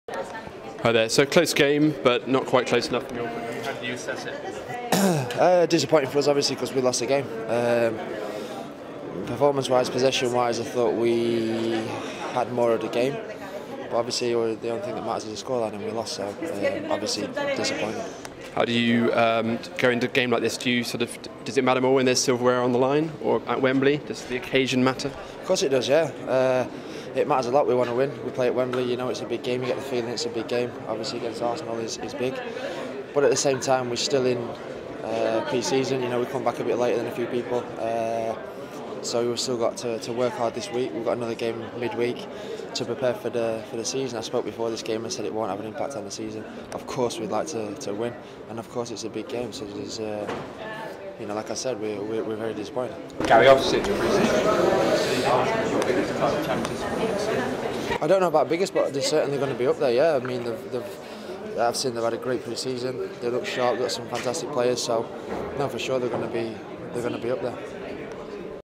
Gary Cahill - I expect Arsenal to be up there
Chelsea defender reflects on Community Shield defeat to Arsenal